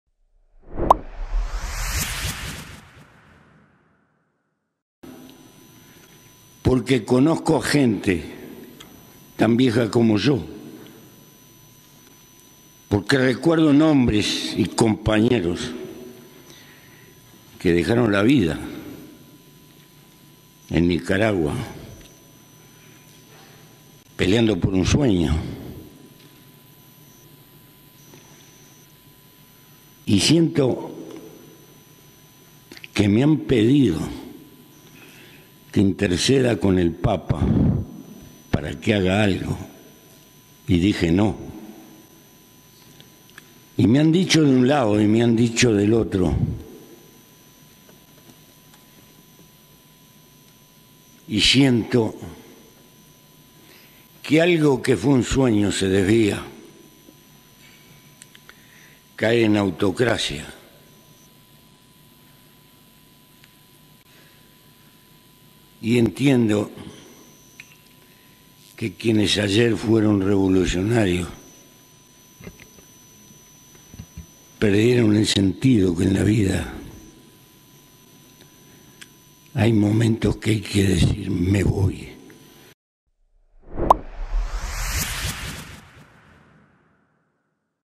Pepe Mujica DECEPCIONADO DEL FSLN y de ORTEGA
"Los que ayer fueron revolucionarios, perdieron el sentido en la vida. Hay momentos que hay que decir: me voy.", refiriéndose al caso de Nicaragua.

Puede visitar el canal de la periodista Jennifer Ortiz, directora de Nicaragua Investiga puedes dar click al siguiente enlace